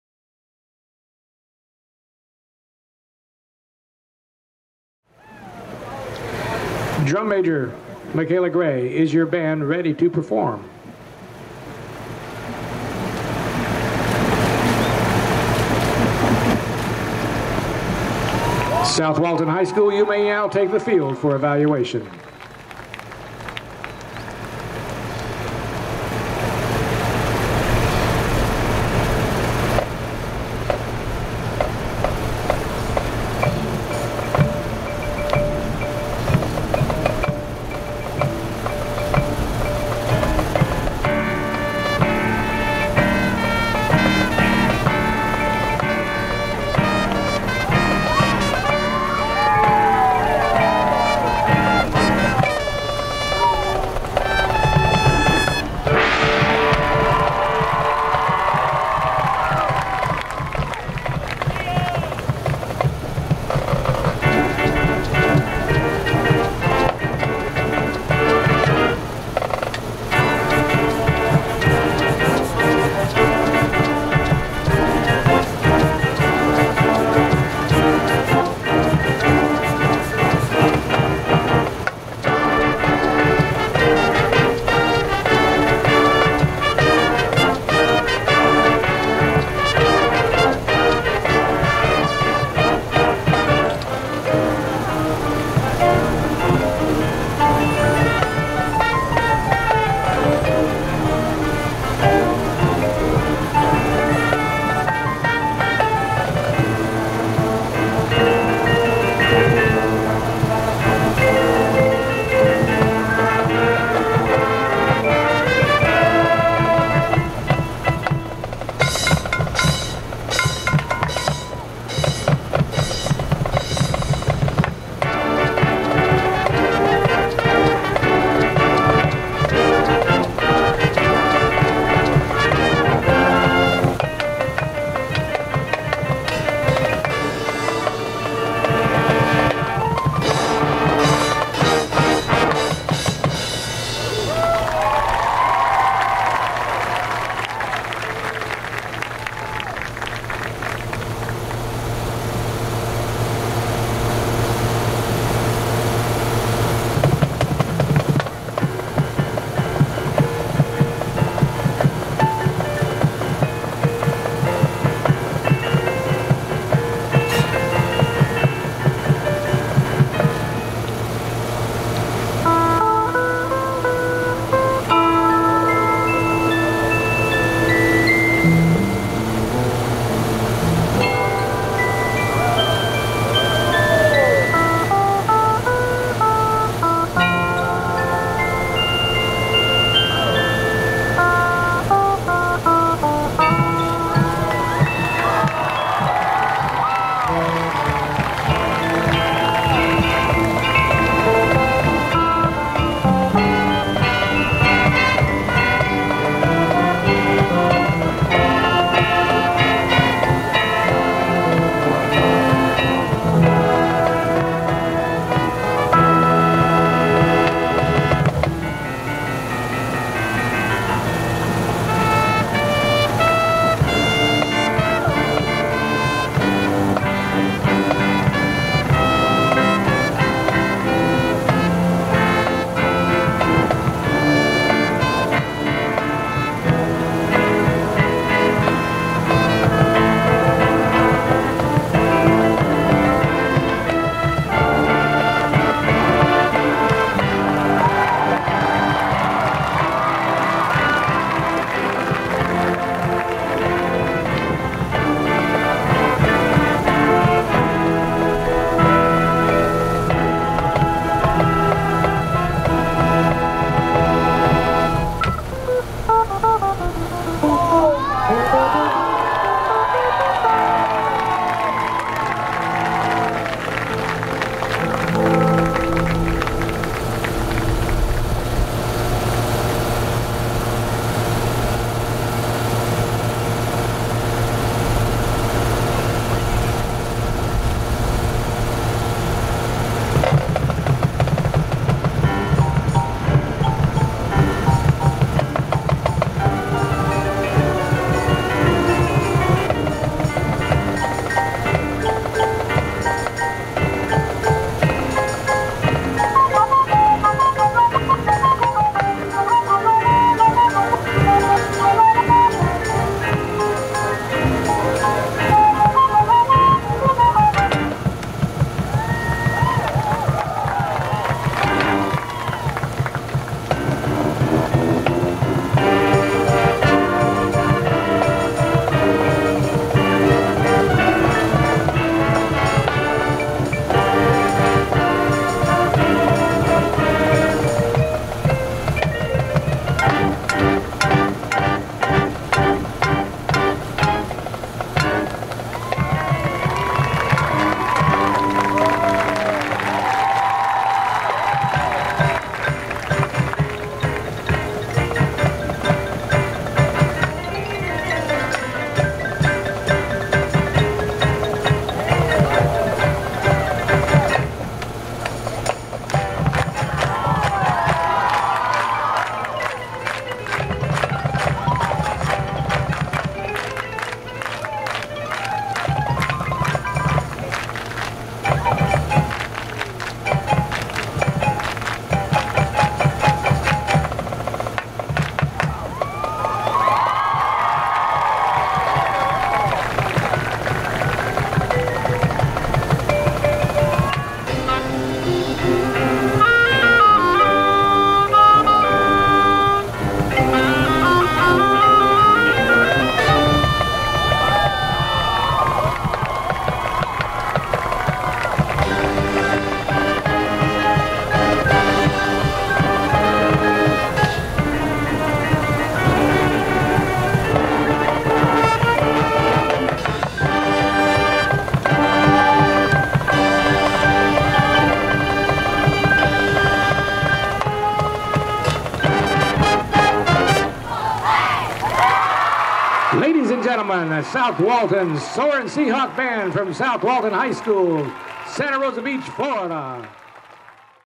South Walton High School Band
Performance at The Blackwater Classic Marching Band Festival held at Milton High School 10/6/18.